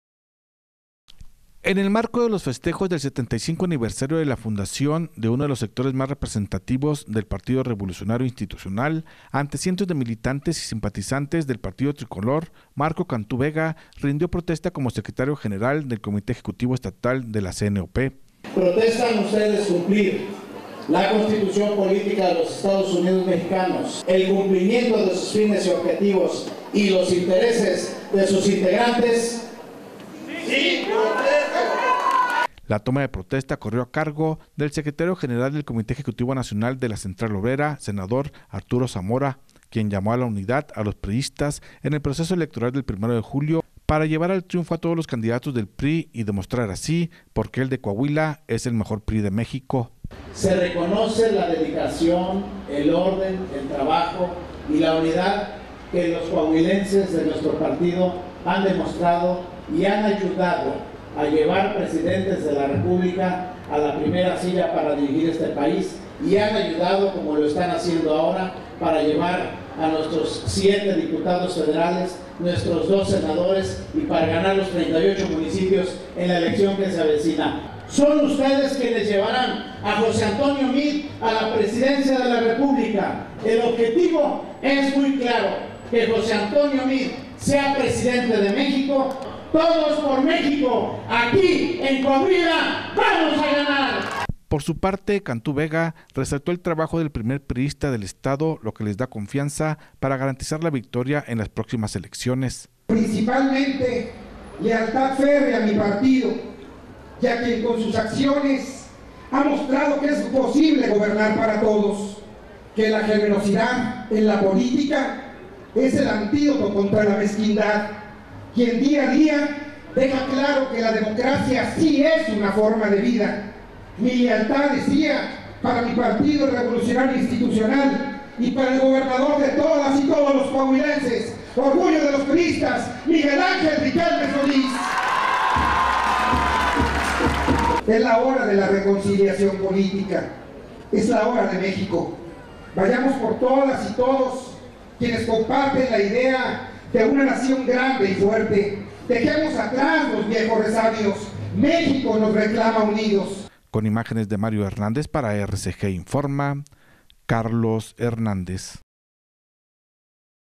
Nuevo Dirigente Tiene la CNOP Coahuila
En el marco de los festejos del 75 aniversario de la fundación de uno de los sectores más representativos del Partido Revolucionario Institucional, ante cientos de militantes y simpatizantes del partido tricolor, Marco Cantú Vega rindió protesta como Secretario General de Comité Ejecutivo Estatal de la CNOP.

La toma de protesta corrió a cargo del Secretario General del Comité Ejecutivo Nacional de la Central Obrera, Senados Arturo Zamora quien llamo a la unidad a los priistas en el proceso electoral del 1º de julio, para llevar al triunfo a todos los candidato del PRI, y demostrar así, porque el de Coahuila, es el mejor PRI de México.